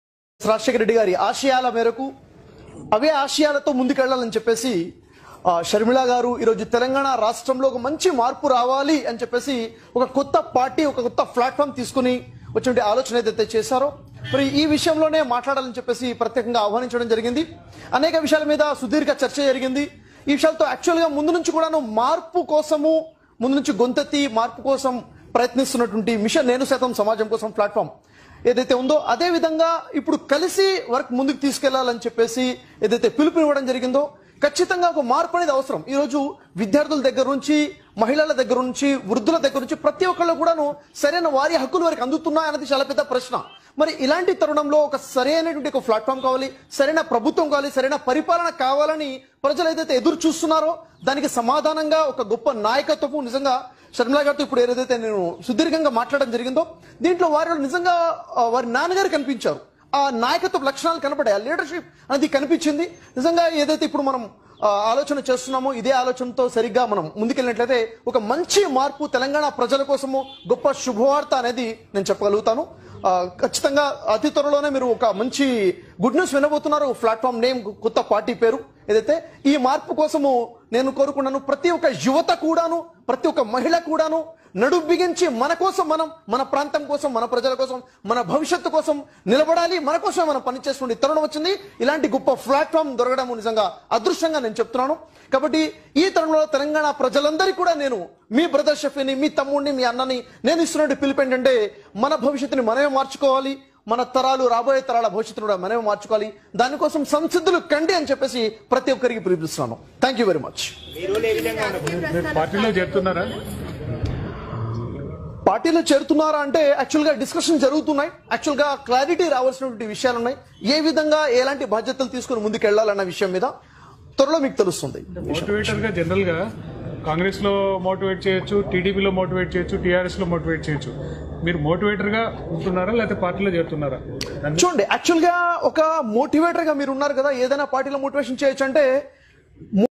లోటస్ పాండ్ లో వైయస్ షర్మిల గారిని కలిసి మద్దతు తెలిపిన మోటివేషనల్ స్పీకర్ Br షఫీ గారు.
Twitter లోటస్ పాండ్ లో వైయస్ షర్మిల గారిని కలిసి మద్దతు తెలిపిన మోటివేషనల్ స్పీకర్ Br షఫీ గారు.